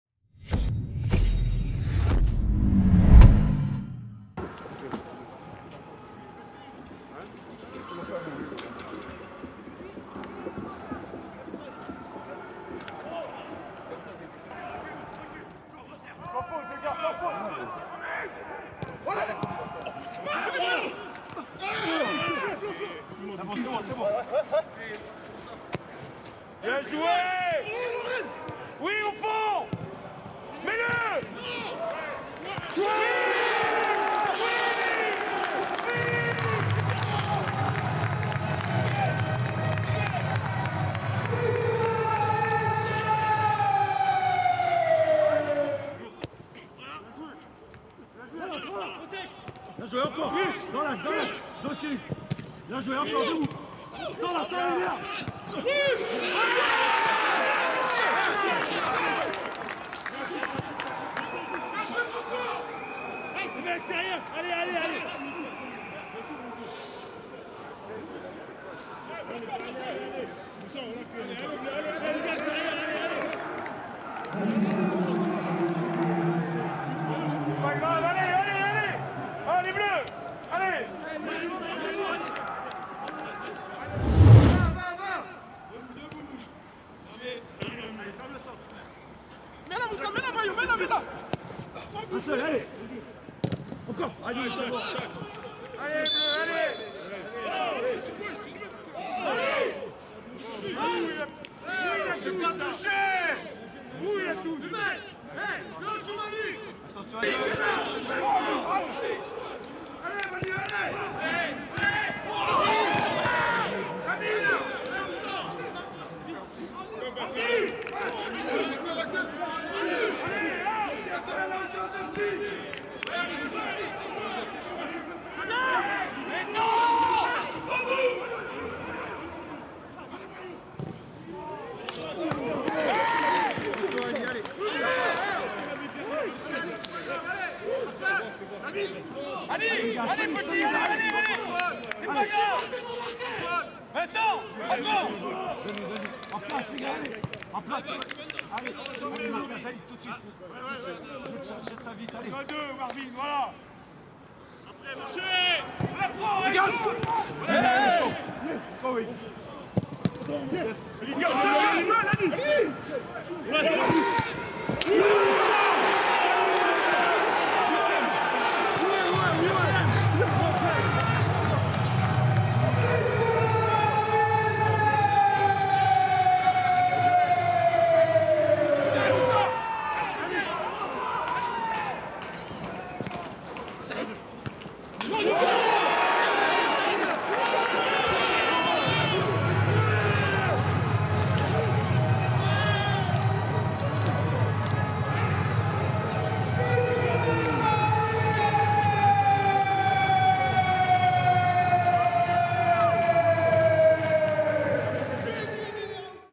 PFCTFC Le résumé de Paris FC/TFC, 36ème journée de Ligue 2 BKT
Les Violets ont été tenus en échec sur la pelouse du Paris FC (3-1). Une issue qui s'est dessinée dans les derniers instants du match, malgré des occasions qui auraient pu permettre aux Toulousains de l'emporter. Revivez le résumé de la rencontre.